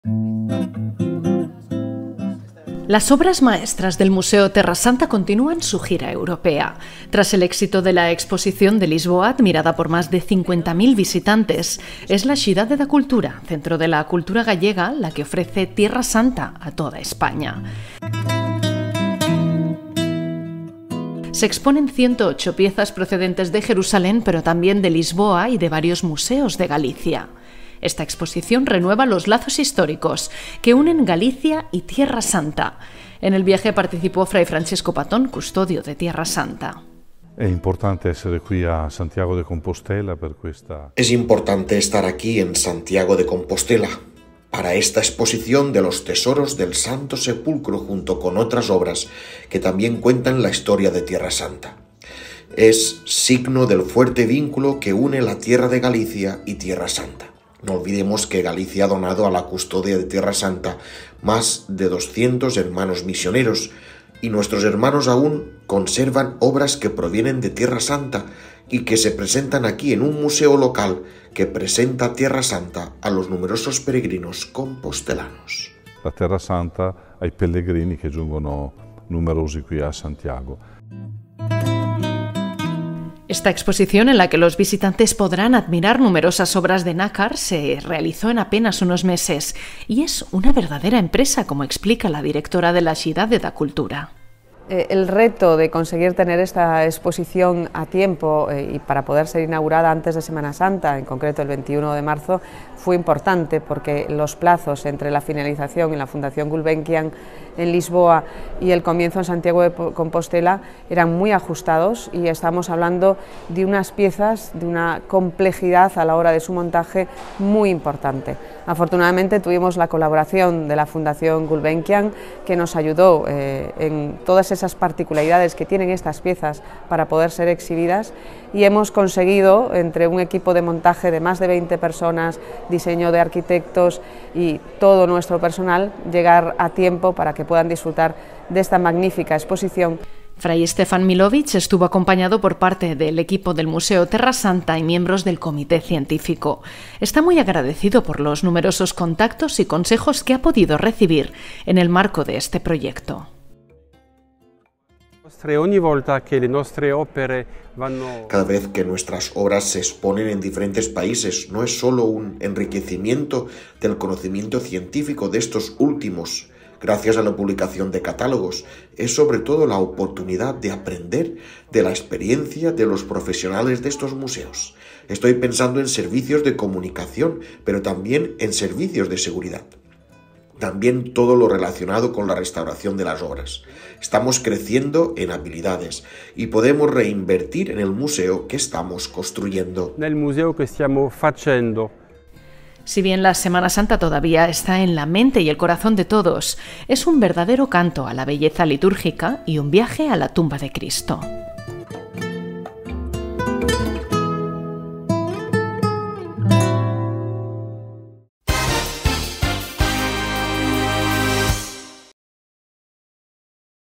Como un perfume de Jerusalén a Compostela
Las obras maestras del Museo Terra Sancta continúan su gira europea. Tras el éxito de la exposición de Lisboa, admirada por más de 50.000 visitantes, es la Cidade da Cultura, centro de la cultura gallega, la que ofrece Tierra Santa a toda España. Se exponen 108 piezas procedentes de Jerusalén pero también de Lisboa y de varios museos de Galicia. Esta exposición renueva los lazos históricos que unen Galicia y Tierra Santa. En el viaje participó fray Francesco Patton, custodio de Tierra Santa.